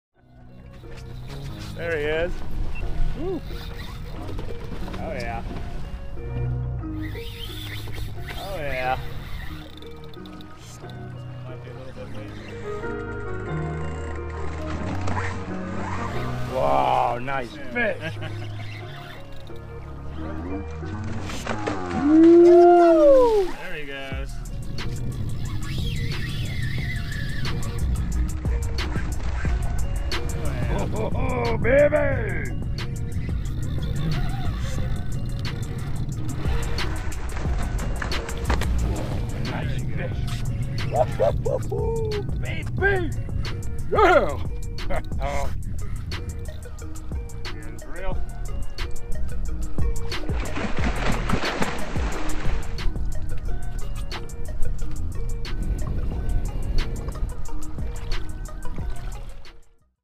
Irv battling a tarpon!
Irv doing battle with a tarpon fishing in Islamorada. March is a great time to find Backcountry tarpon away from the crowds.